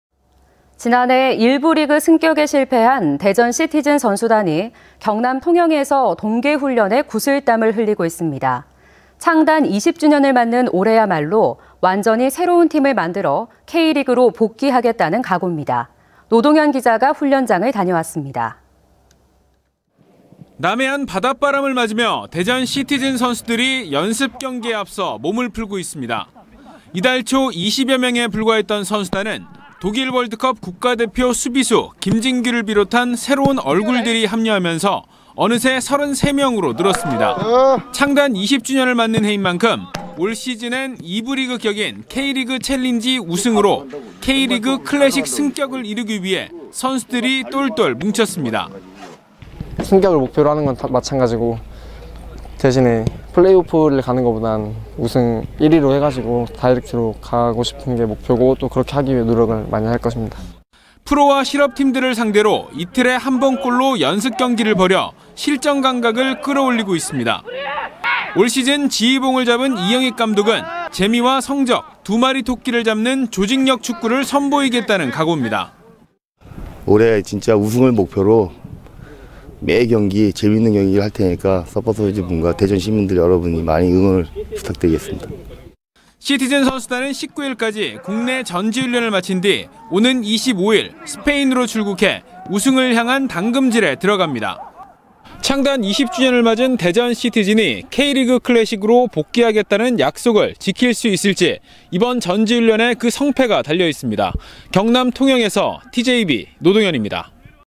[TJB뉴스] 대전시티즌 1부리그 복귀 '구슬땀'
【 앵커멘트 】
지난해 1부리그
승격에 실패한
대전시티즌 선수단이
경남 통영에서
동계훈련에 구슬땀을
흘리고 있습니다.

창단 20주년을 맞는
올해야말로 완전히
새로운 팀을 만들어
K리그로 복귀하겠다는
각옵니다.

노동현 기자.